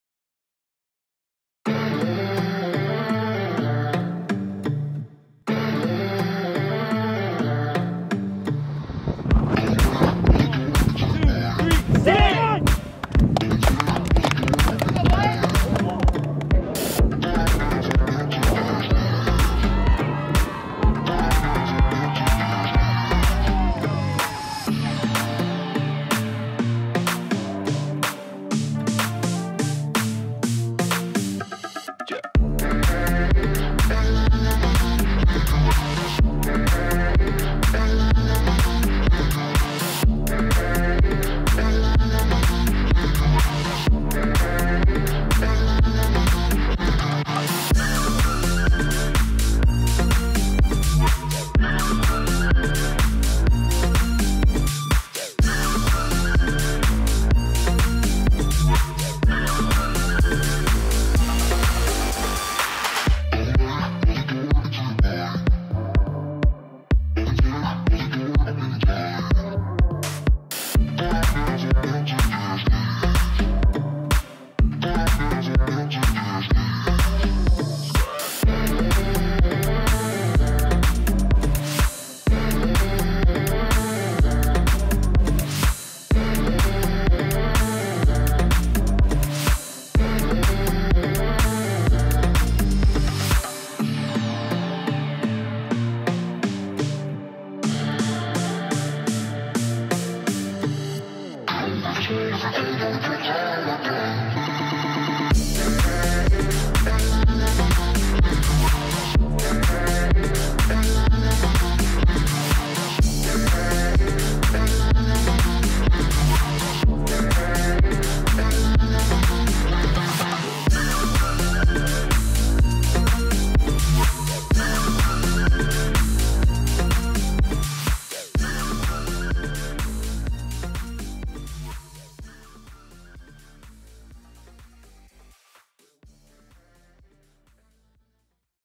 Sting Men's Cross Country @ Centennial Invitational - Oct.16.2021
Highlights from the OCAA Men's 8km Cross Country race hosted by Centennial College. Jonah Bisgould-Menendian was the top runner for the Sting finishing 24th. The race took place on October 16, 2021 at Ashtonbee Reservoir Park.